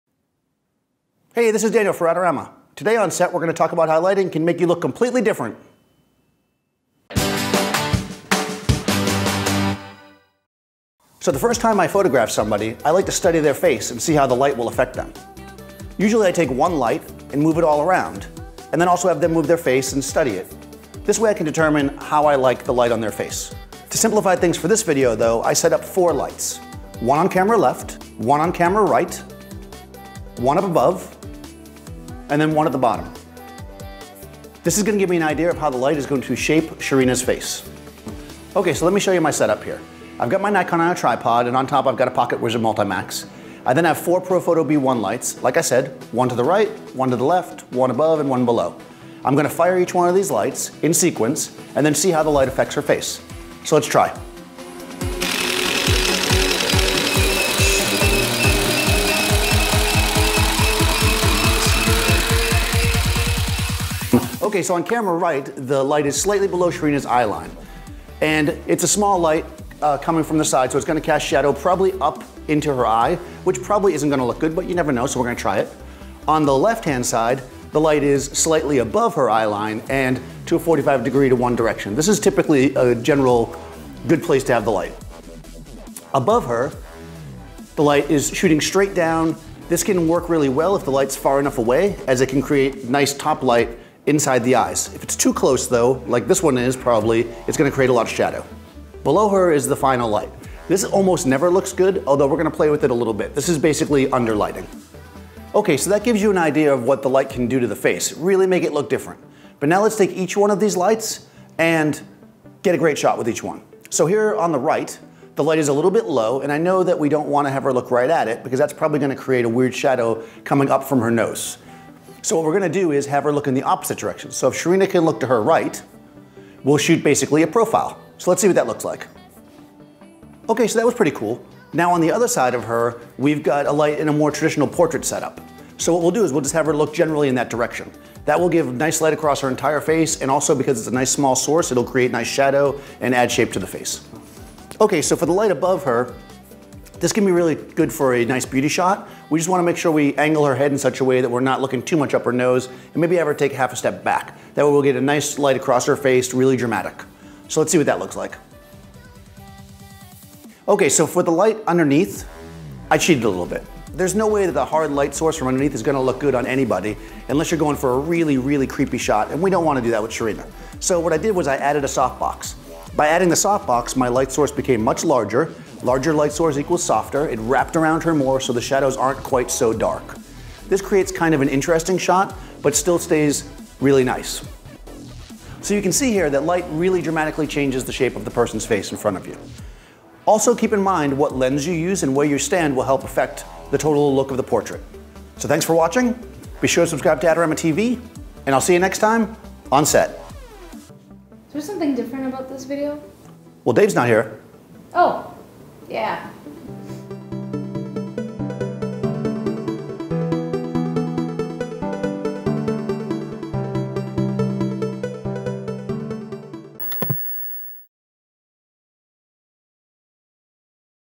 4 Simple Ways To Light the Face - OnSet ep. 74
Join Daniel Norton OnSet as he demonstrates the dramatic changes lighting position can have on your subject's face.

Photos by Daniel Norton

You can keep up with Daniel on Instagram @danielnorton